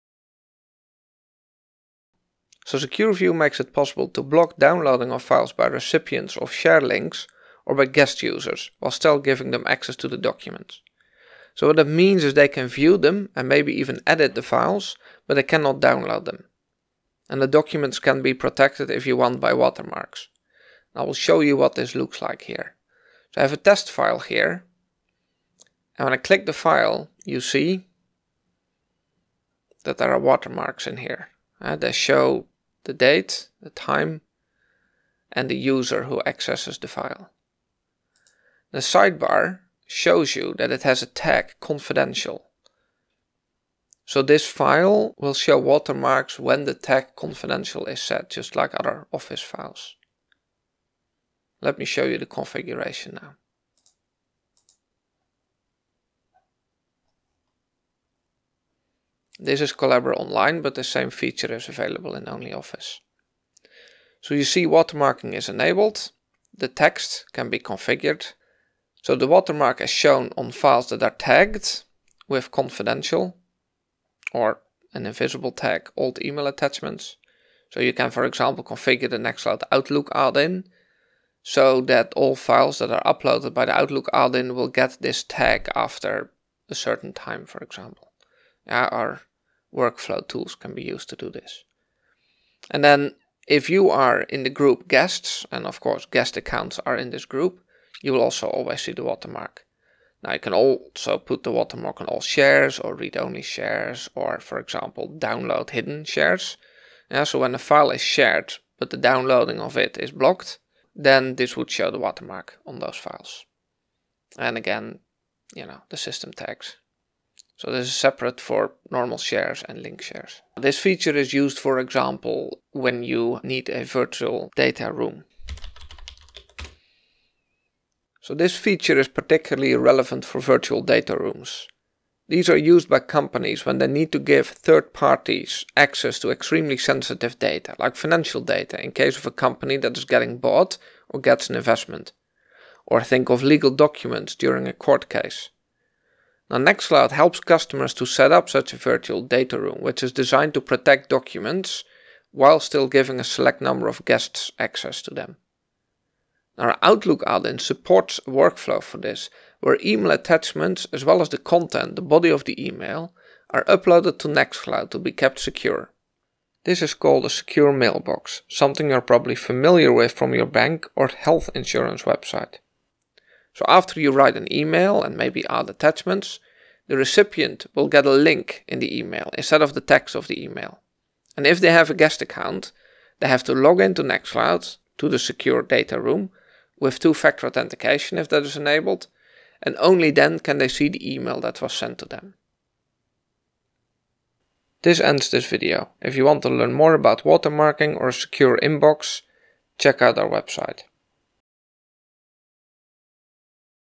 Secure view: blocking downloads, watermarking and secure mailboxes in Nextcloud
Nextcloud 15 introduced the ability to block downloads, expanded into our secure view feature since. It is now possible to ensure documents are only shown with watermarks to guests.
Meanwhile, our Outlook add-in added a secure mailbox feature which protects the mail body and attachments from leaking!
Together, these functions are part of our virtual data room offering for customers.